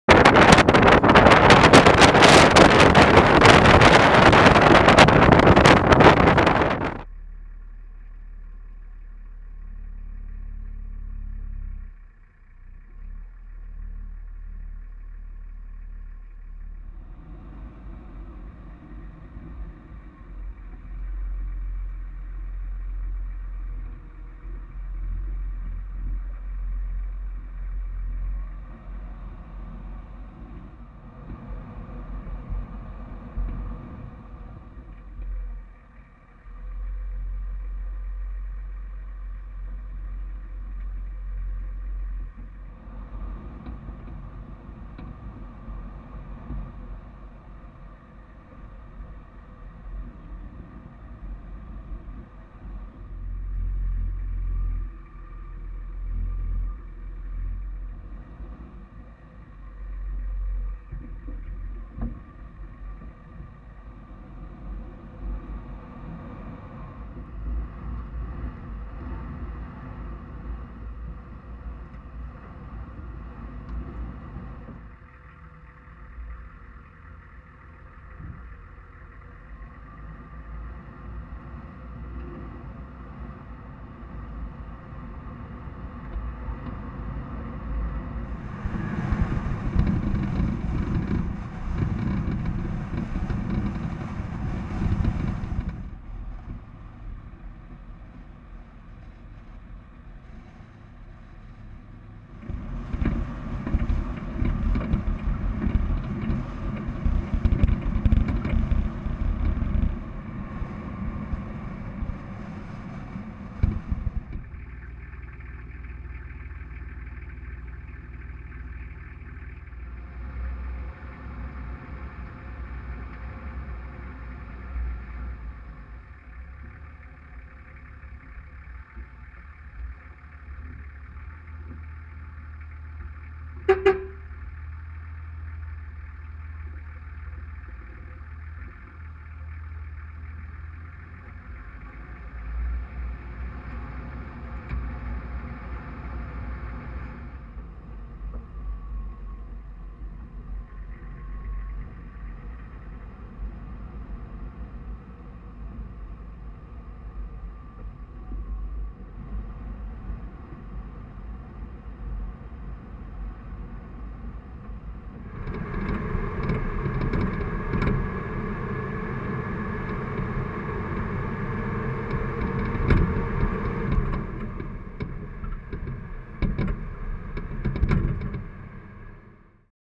Chimborazo 4x4 Tour
Some backrouds at Chimborazo area on our overland trip with our Toyota HZJ75 through the americas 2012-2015.